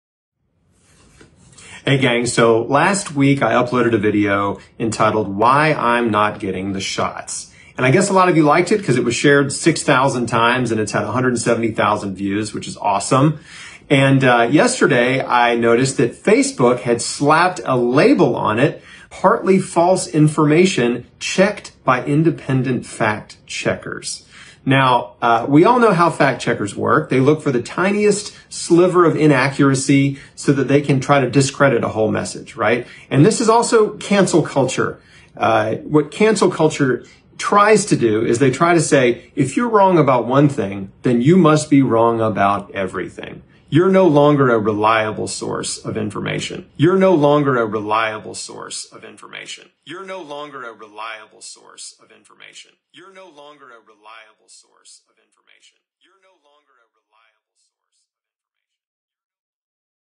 Chris Wark fact-checked by Facebook, again.
The two ChrisBeatCancer Facebook videos which have a fact-check warning are ...

Without these warnings some viewers would be completely unaware the pied piper is leading them off a cliff, as dissent is screened-out of the associated comments.

Apparently there is a directors-cut of "Why I'm not getting the shots" on Rumble ...